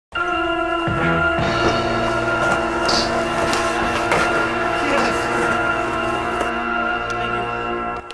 kickflip
me kickfliping the 2 flat 2 at kona